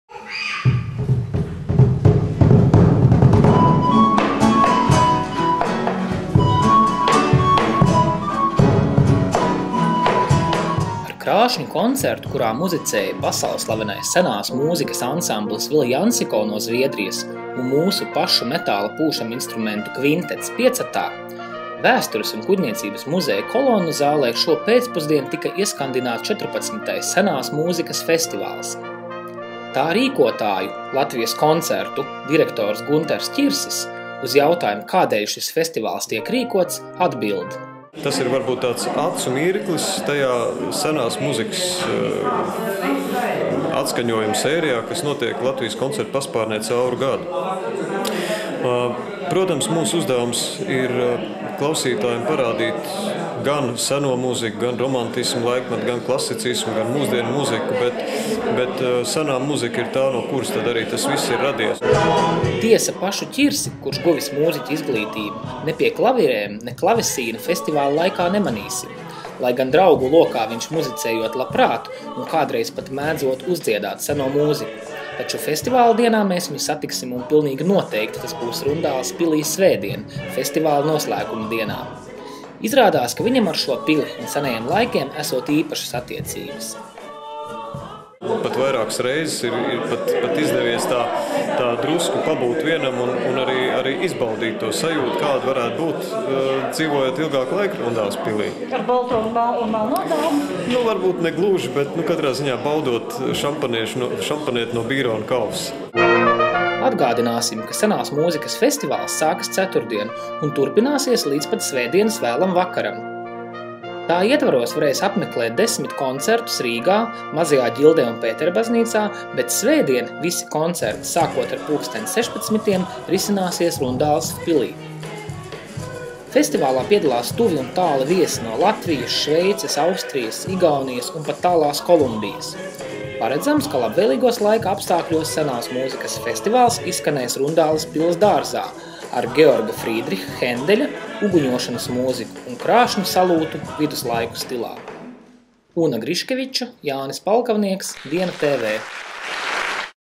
Ieskandināts Senās mūzikas festivāls
Ar krāšņu koncertu, kura muzicēja pasaulslavenais senās mūzikas ansamblis "Villancico" no Zviedrijas un mūsu pašu pūšaminstrumentu kvintets "5atā", tika ieskandināts XIV Senās mūzikas festivāls.